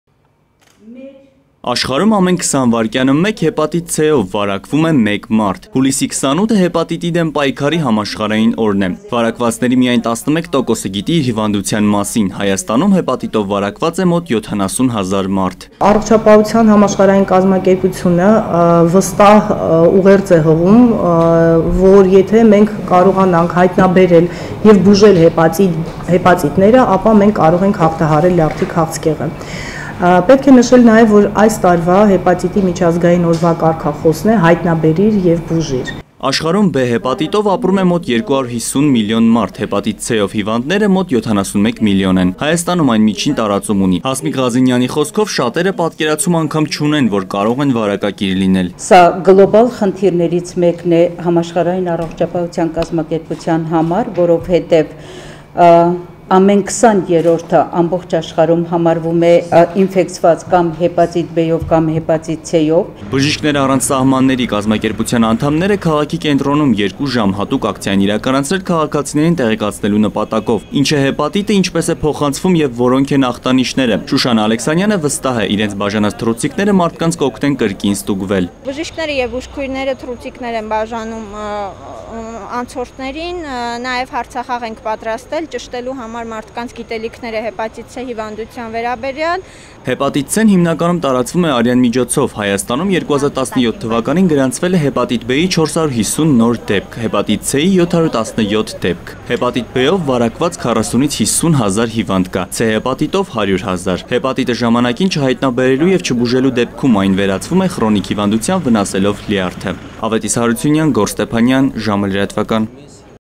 Հեպատիտի դեմ պայքարի օր. ՀՀ բոլոր կլինիկաներում հնարավոր է ստուգել Հեպատիտի վարակը
Հայաստանում հեպատիտով վարակված է մոտ 70 հազար մարդ: